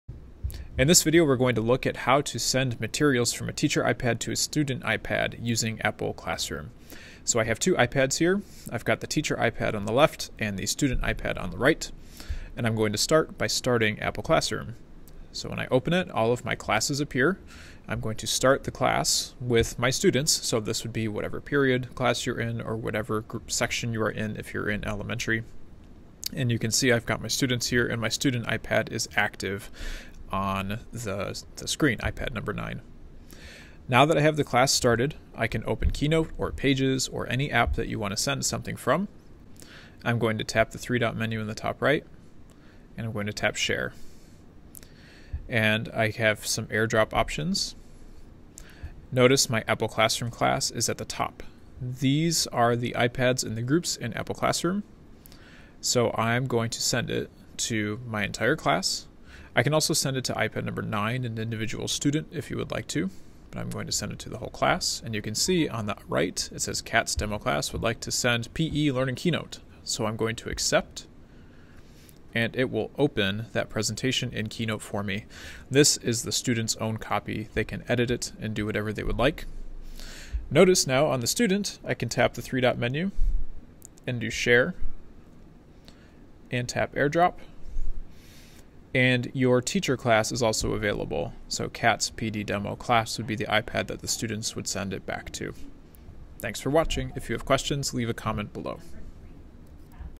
Share Materials Through Apple Classroom
Apple Classroom allows teachers and students to share materials back and forth using AirDrop. This side-by-side video shows a teacher and student view at the same time (it is 2020, after all) demonstrating how to use Apple Classroom to make the impossible possible.